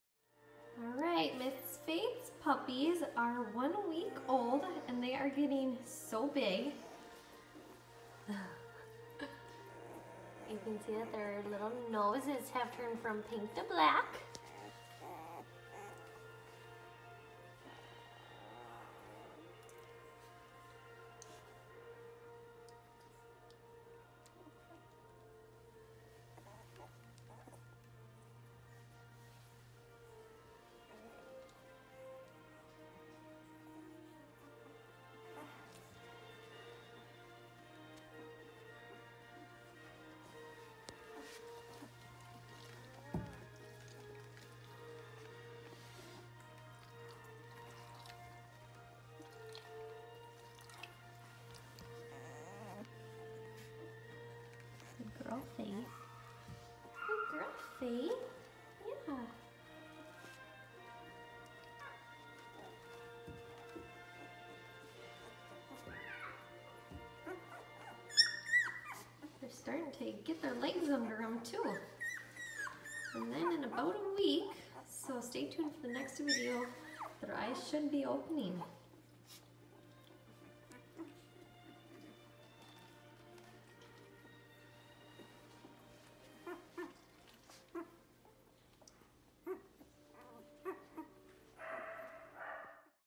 Faith 1-week-old British Labrador Puppies
Craighorn Faith
x Minstead Delvin (a.k.a. Del)
Whelp Date: Monday October 8, 2018.
10 Puppies: 3 Black Males, 3 Black Females, 2 Yellow Males, 2 Yellow Females